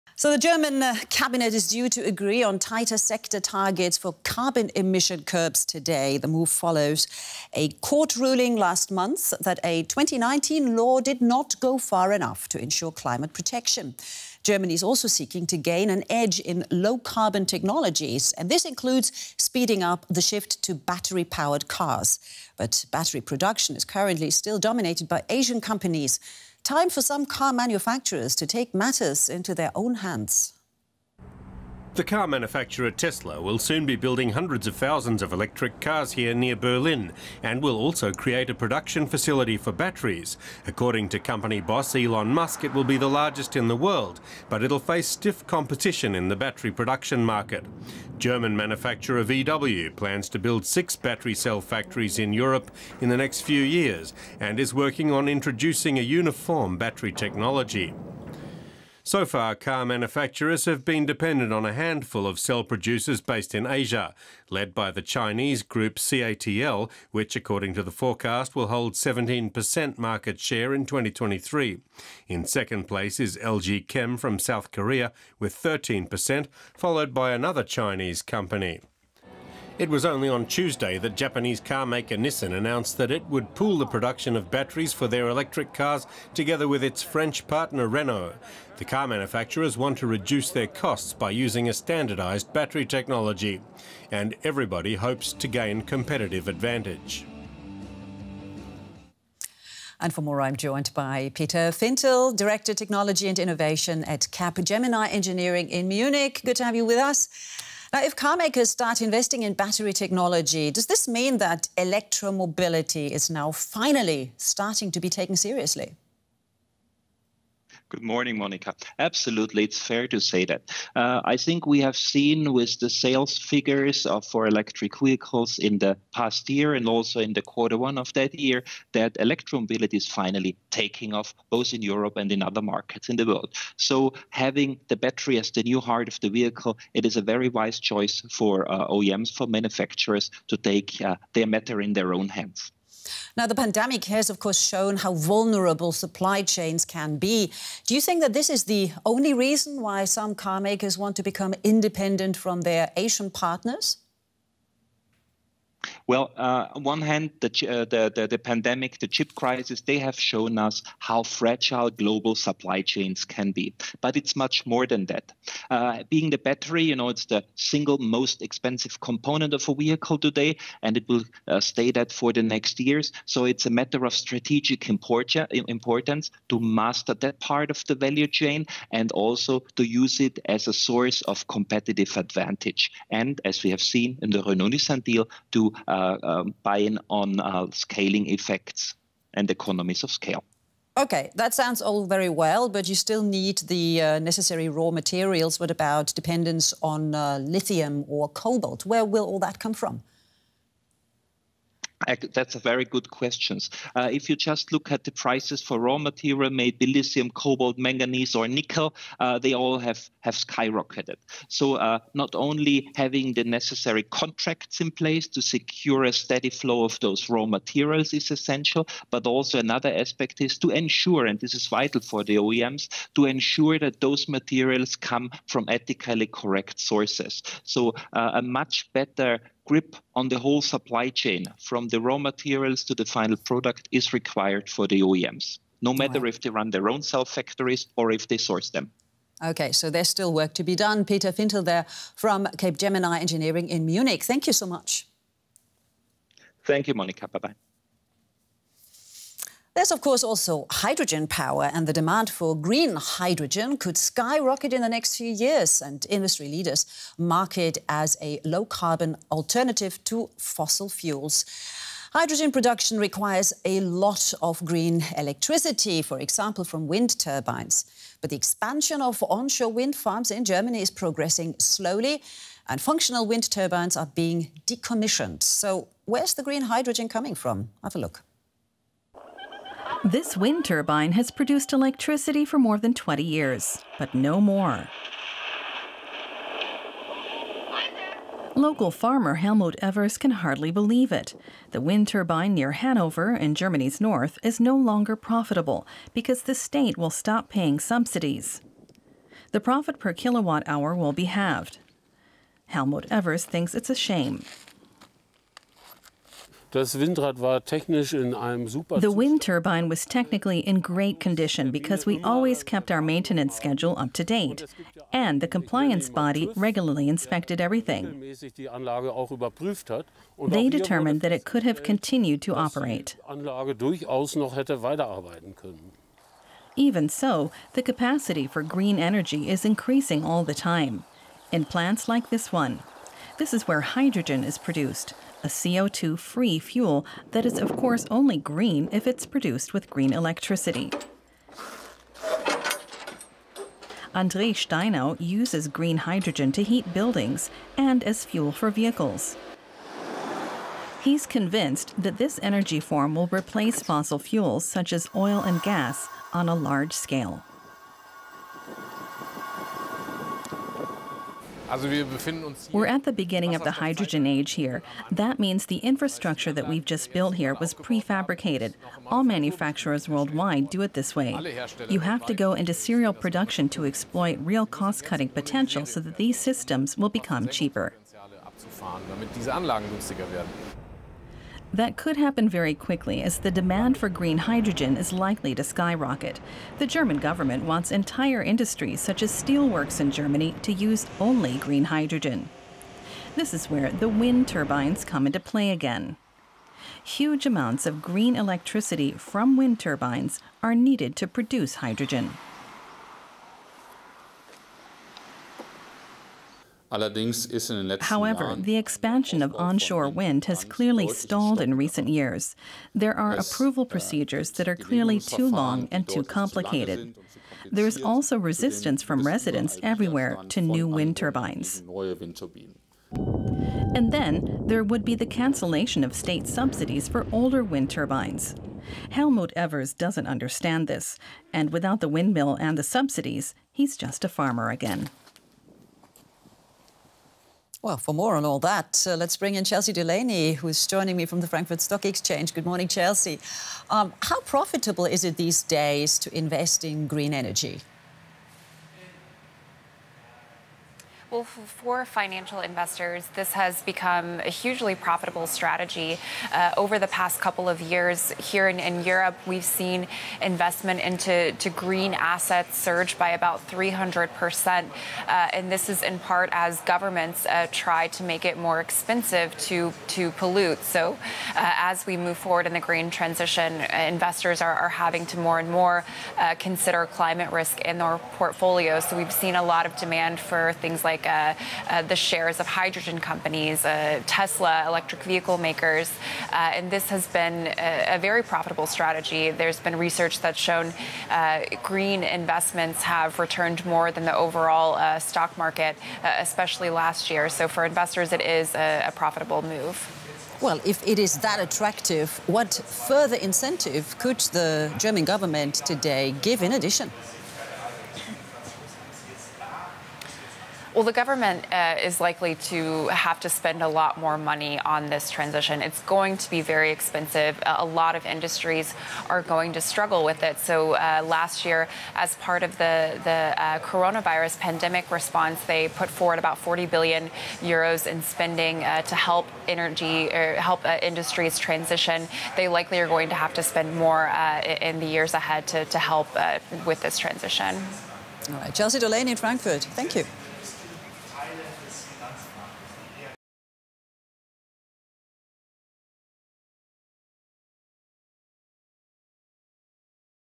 Electric batteries, fuel cells, hydrogen fuel: Carmakers look for energy solutions | DW News
No car manufacturer can avoid electromobility. After long start-up difficulties, everyone has made this a priority. In order to achieve the climate protection goals (see the occasion today) and not least because China, the important car sales market, is demanding more electrical outlets. Batteries are needed for e-cars. However, their production is currently still dominated by Asian companies that do not necessarily build cars. In view of the rapidly growing e-fleets, some car manufacturers want to take this into their own hands
At the same time the demand for green hydrogen will skyrocket in the next few years. This requires a lot of green electricity, for example from wind turbines. But the expansion of onshore wind farms is only progressing slowly. And at the same time, functional wind turbines are even being demolished.
Where will the electricity come from to switch to green hydrogen?